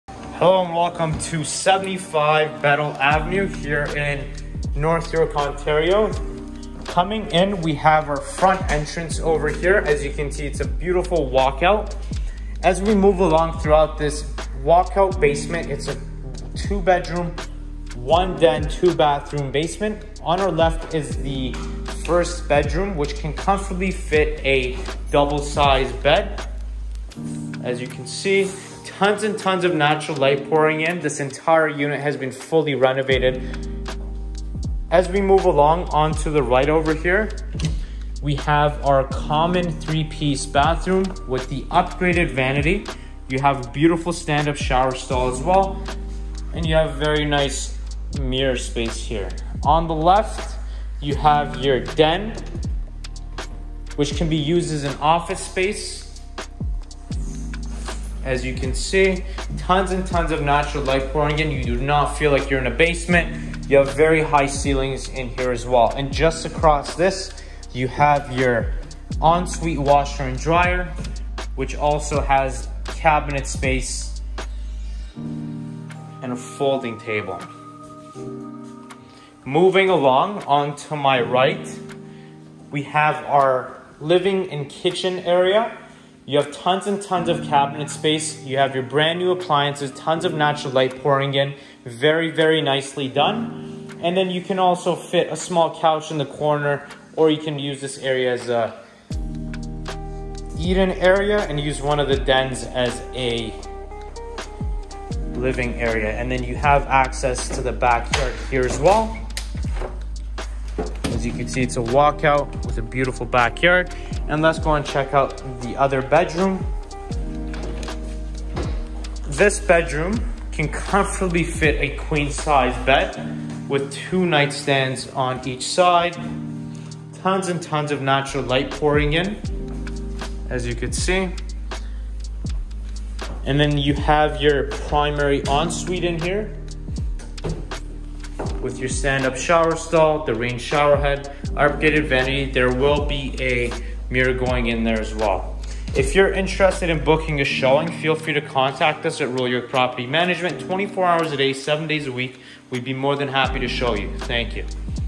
2-75 Bedle Avenue, North York, Ontario M2H 1K7
2 Bedrooms + DEN | 2 Bathrooms, Upgraded Kitchen, Back Splash, White Appliances, Dishwasher, Stone Countertops, Ensuite Laundry, 9 Foot Ceilings, Window Coverings, Tons of Natural Light.

Close to Highway 404, Don Mills, North York Centre, McNicoll Avenue at Bedle Avenue, Don Mills Road / Bedle Avenue, Hillmount Public School, Cresthaven Public School, Duncan Creek Park, McNicoll Park, Skymark Place Shopping Centre, Cliffwood Plaza, North York Zion Schoolhouse, Woodbine Beach.

2-75 Bedle Avenue, North York, Ontario M2H 1K7
Don Mills Road and Finch Avenue East

20 Minutes Away From the Beautiful Woodbine Beach!!
Located in Beautiful Neighborhood!!

$1,750 (One Parking Space Included)
Discounted Cable & Phone (Throughout the Term, at Additional Cost)

UNIT AMENITIES:
*2 Bedrooms + DEN | 2 Bathrooms
*Upgraded Kitchen
*Upgraded Back Splash
*White Appliances
*Stone Countertops
*Dishwasher
*Vinyl Floors
*9 Foot Ceilings
*Roller Window Coverings
*Ensuite Laundry
*En-Suite Bathroom
*Lawn and Snow Removal Included
*Tons of Natural Light
*This is a Newly Renovated Basement Apartment
*Tenant is Liable for 35% of the Utilities

Available Immediately!!